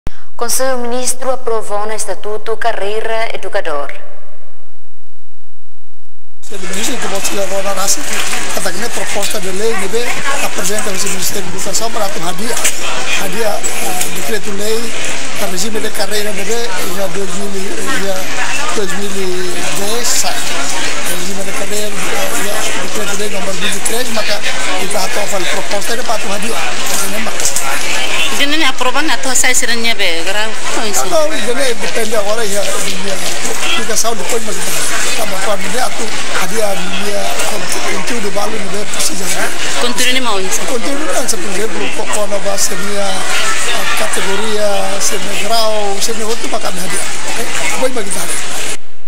KM Aprova Estatutu Kareira Edukador, Hadia Kontiudu Balun
Konsellu Ministru, Aprova Ona Estatutu Kareira Edukador. Husi Estatutu Nee, Kontiudu Balun Presiza Hadia.